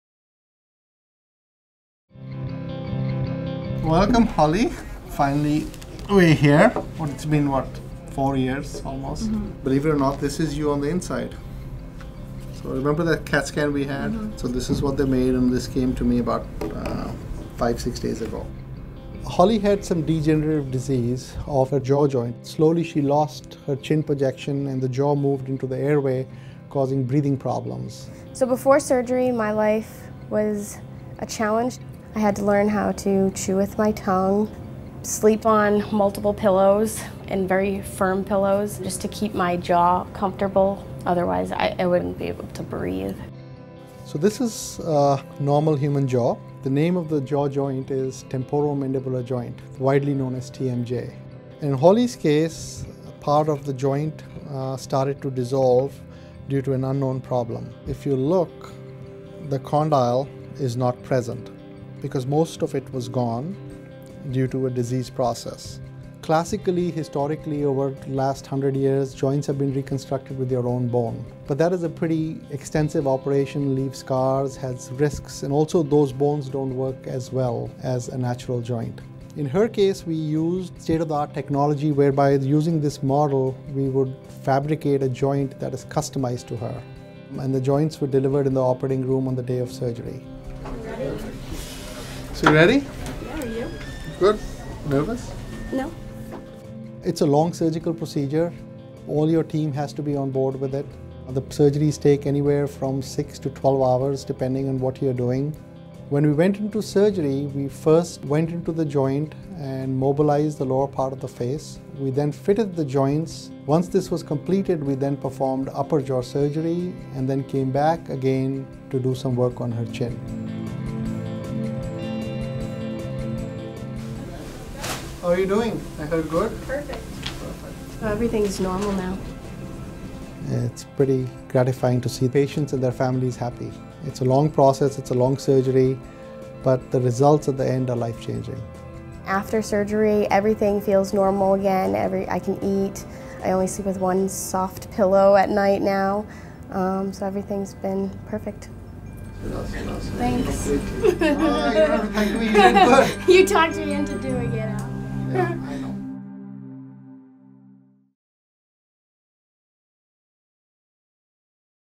Saving Face Through TMJ Surgery
Dr. Pushkar Mehra explains the cutting edge surgery he performs at Boston Medical Center. Mehra's surgery involves a 3d printed skull and custom designed titanium replacement joints.